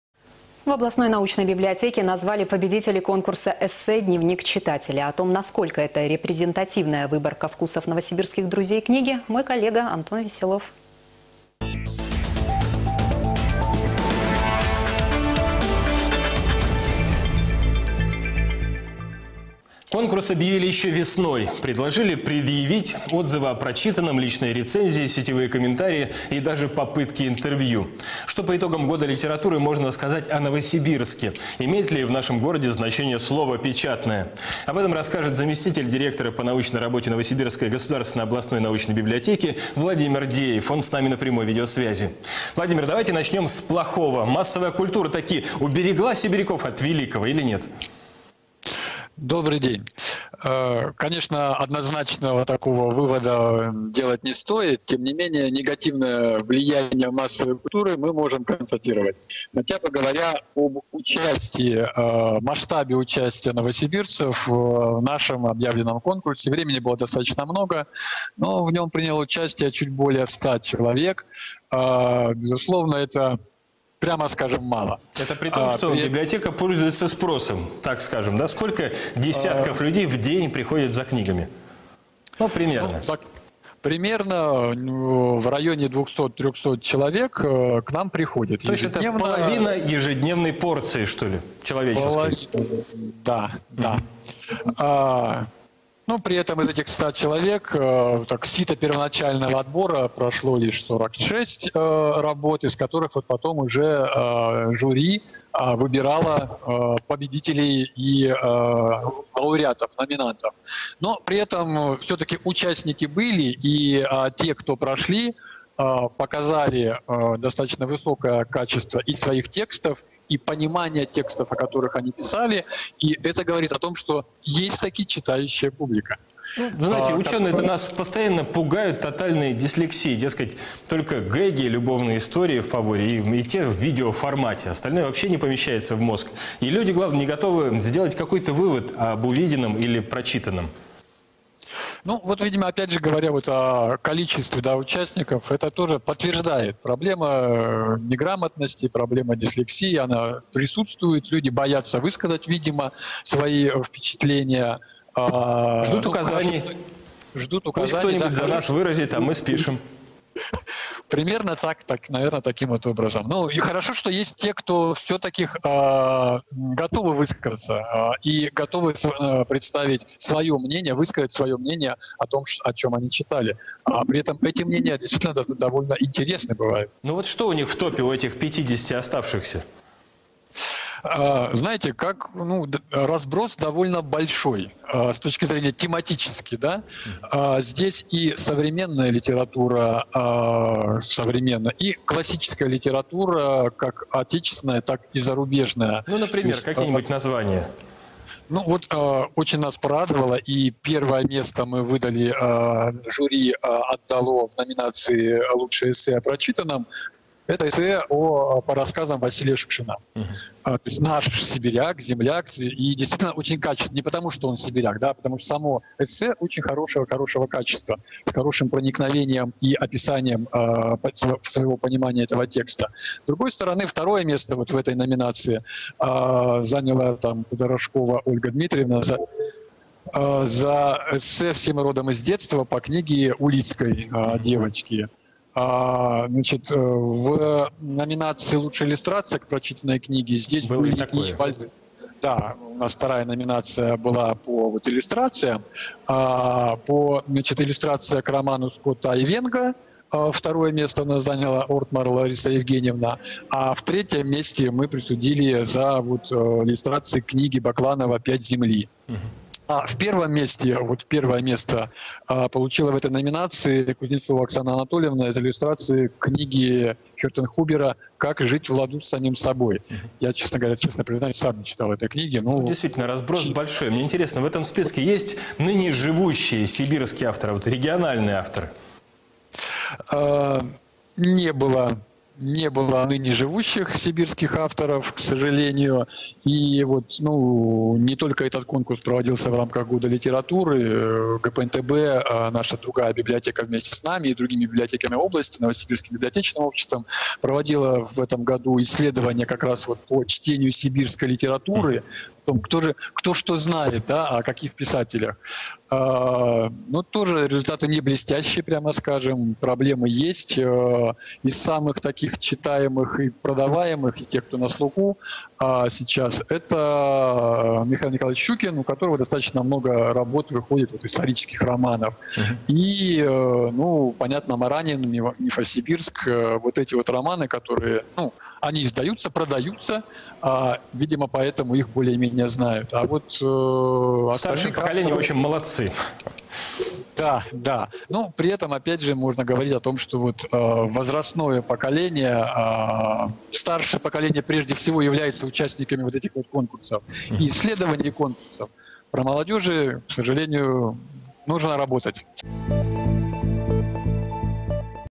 О конкурсе "Дневник читателя" в рамках Года литературы в Новосибирске
В рамках Года литературы-2015 Новосибирская областная научная библиотека проводила творческий конкурс читательских эссе "Дневник читателя". О том, как проходил конкурс, кто принял в нем участие и кто получил награды, рассказывает Владимир Деев, заместителя директора НГОНБ по научной и библиотечной работе.

Интервью в эфире телеканала Вести 24. Новосибирск вел Антон Веселов.